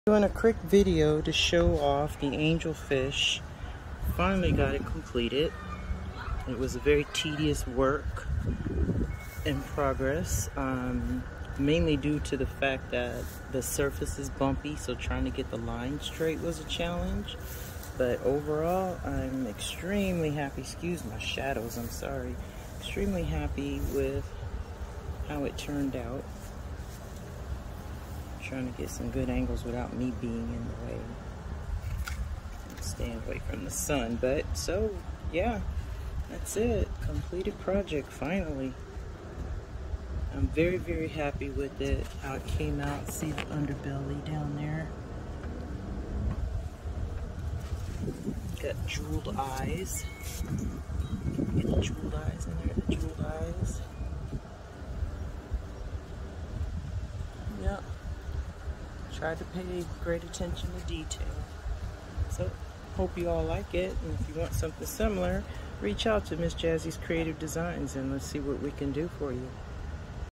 COMPLETED HAND PAINTED ANGEL FISH
Painted a all white ceramic Angel Fish for my clients back patio.